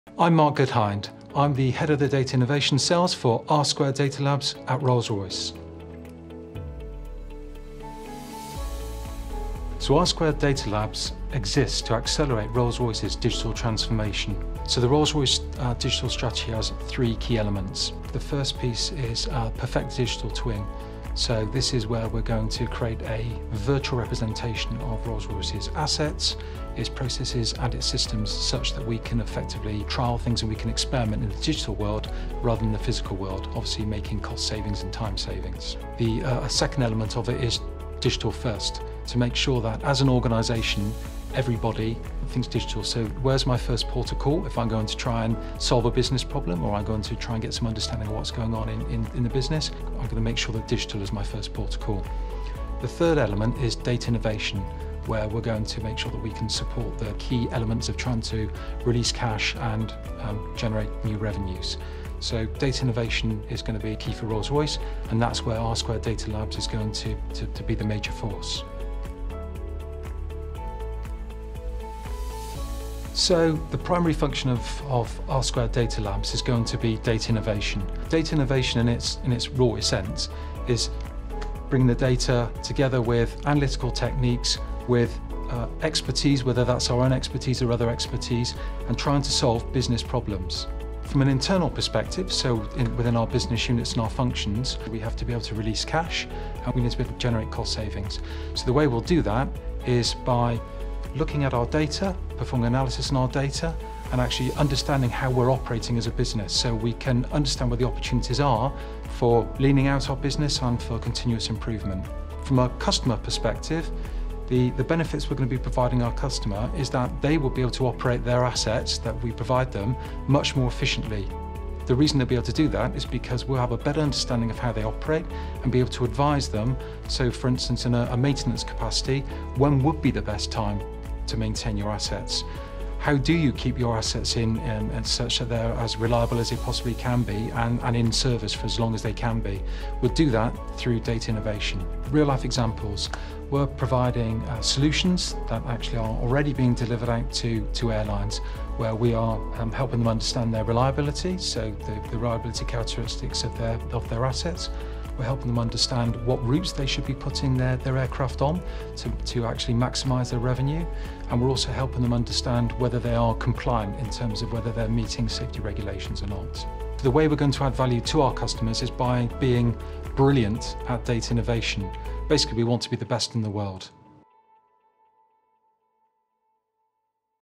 Rolls-Royce | Mark Goodhind on how R² Data Labs creates value through data innovation
Mark Goodhind, Head of Data Innovation Cells, shares how R² Data Labs creates value through data innovation for Rolls-Royce & our customers.

Rolls-Royce is a pre-eminent engineering company focused on world-class power and propulsion systems.